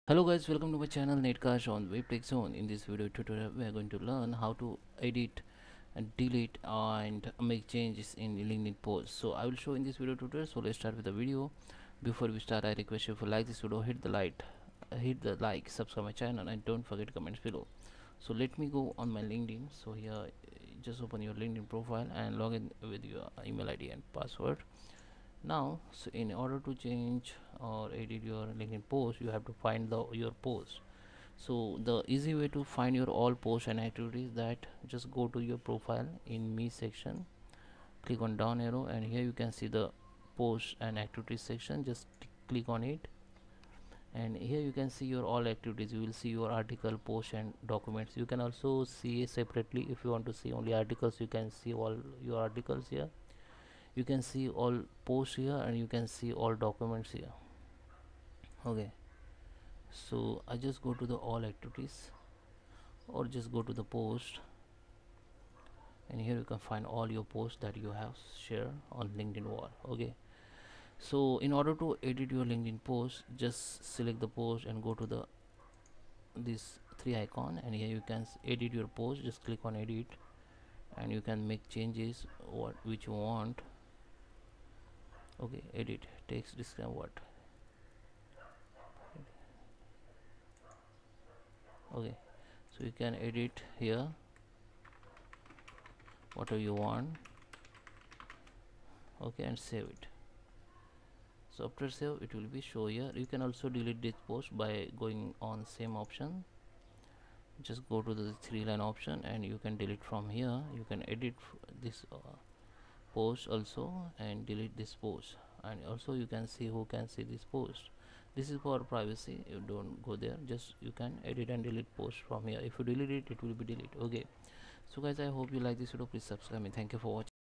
How to Edit,Delete and Make Changes Post in Linkedin
In this video tutorial, we will learn how to edit,delete and make changes post in linkedin and how to edit post in linkedin.